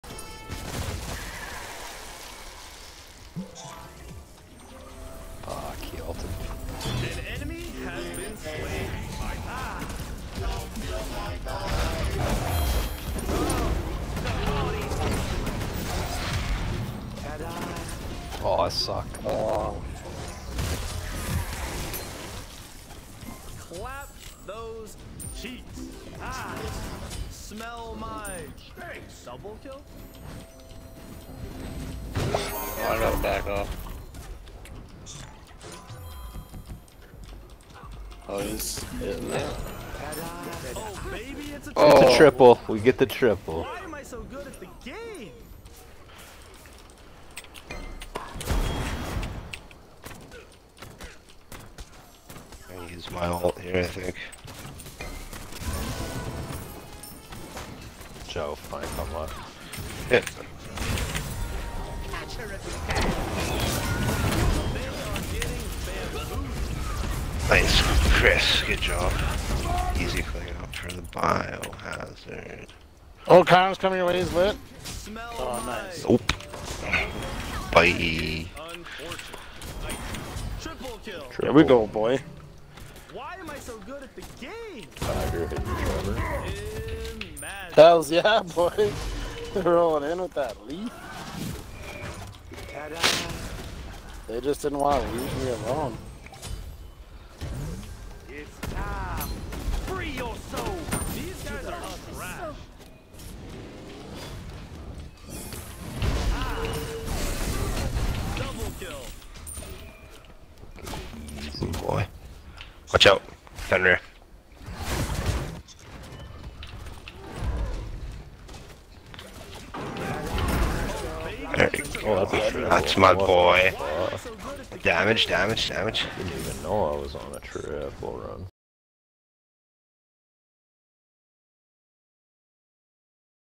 Delayed Triples on the old map
Going through the old archive (clips from back in February) and found some footage from the old map where I accomplished some delayed triple kill clips.  Nothing too special and could not have achieved them without the boys Mukumid420 & cdt Shades setting me up.